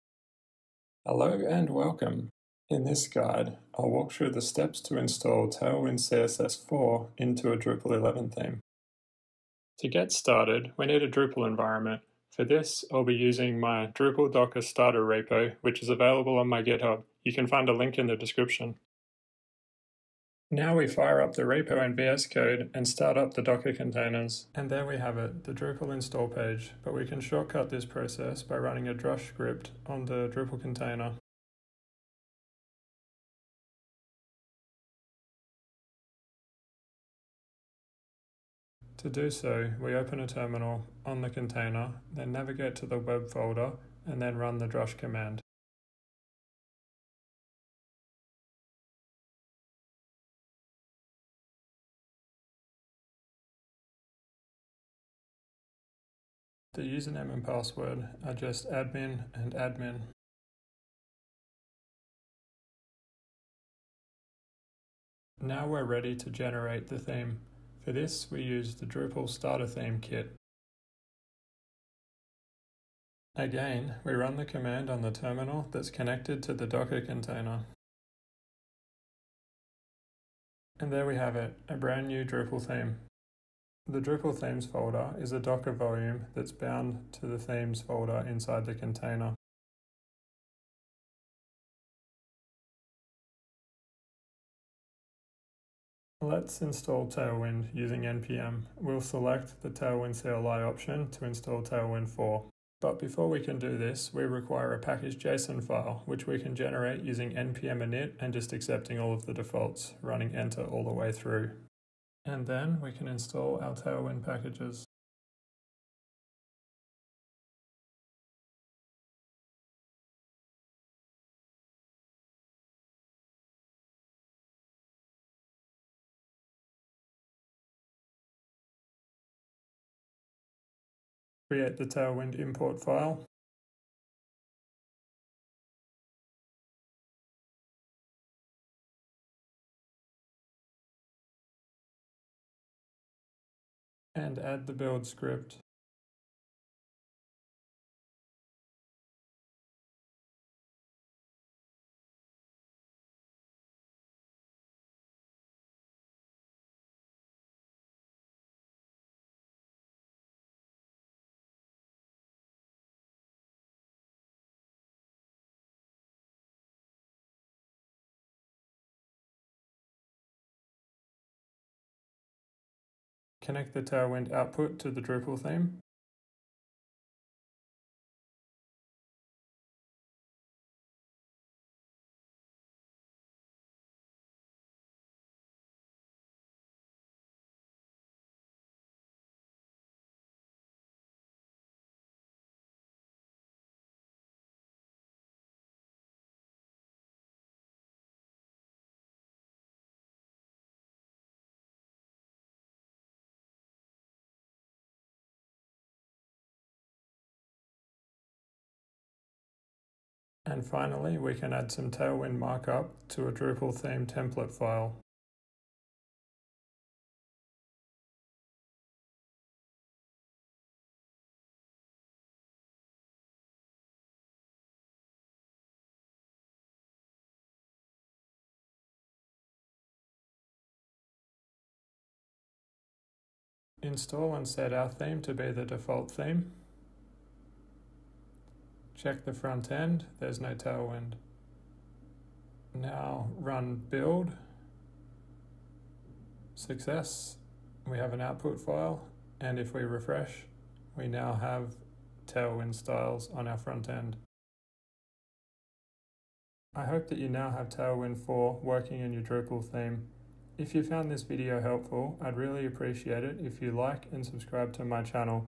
Tutorial: Add Tailwind CSS 4 to your Drupal 11 Theme
In this guide, I’ll walk through the steps to install Tailwind CSS 4 into a Drupal 11 theme.